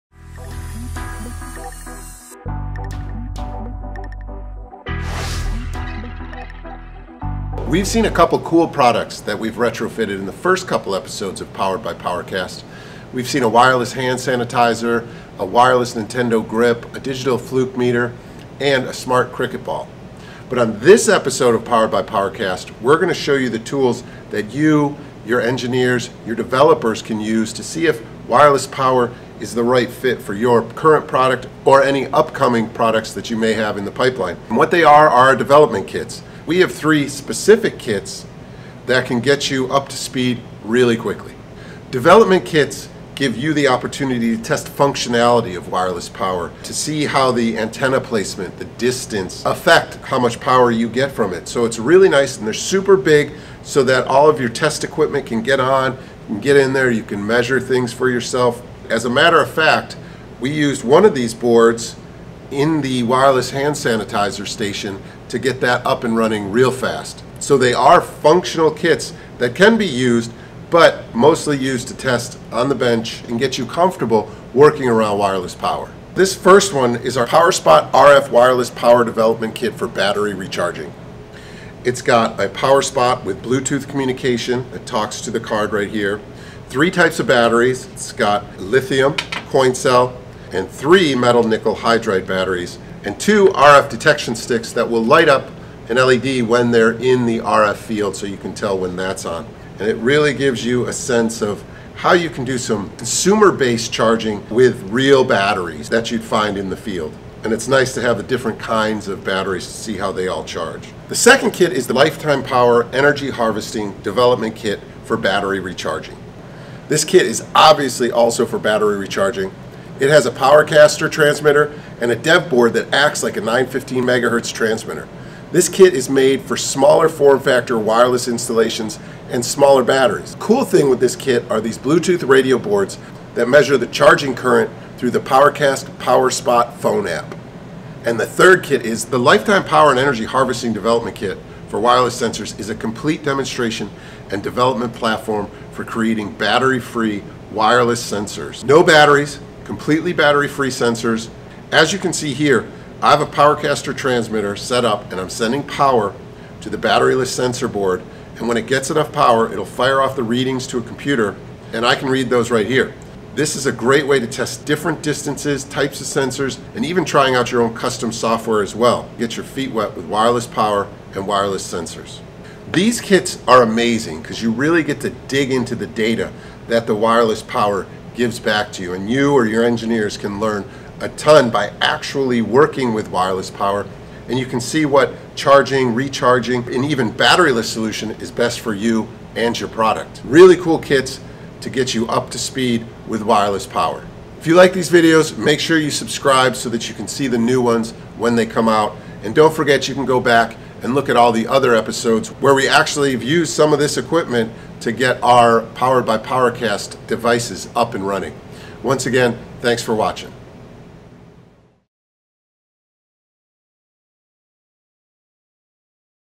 Powered by Powercast - Episode 6 - Development Kits
Development kits are designed so you can easily test and learn about all ins-and-outs of RF Wireless Power. The kits come with transmitters and multiple boards to test everything involved with RF wireless Charging and Wireless Sensors.

We have 3 development kits available for purchase:
1) PowerSpot® RF Wireless Power Development Kit for Battery Recharging
2) Lifetime Power® Energy Harvesting Development Kit for Battery Recharging
3) Lifetime Power® Energy Harvesting Development Kit for Wireless Sensors (jointly developed by Powercast and Microchip)